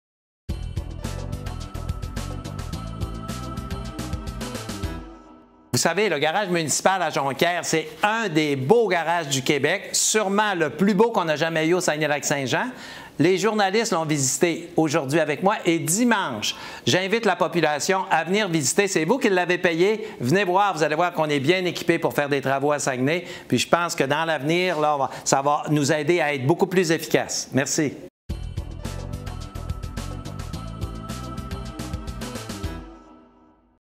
Journée porte-ouverte au nouveau garage municipal
Le maire de Saguenay invite la population à venir visiter le nouveau garage municipal de l'arrondissement de Jonquière, dans le cadre d'une journée porte-ouverte le dimanche 4 juin 2017.